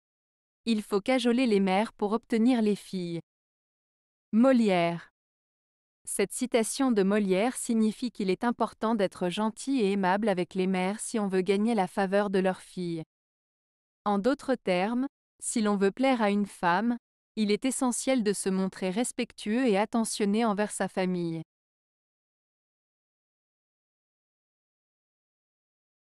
Il faut cajoler les mères pour obtenir les filles. - Molière
Cette citation de Molière signifie qu'il est important d'être gentil et aimable avec les mères si on veut gagner la faveur de leurs filles. En d'autres termes, si l'on veut plaire à une femme, il est essentiel de se montrer respectueux et attentionné envers sa famille.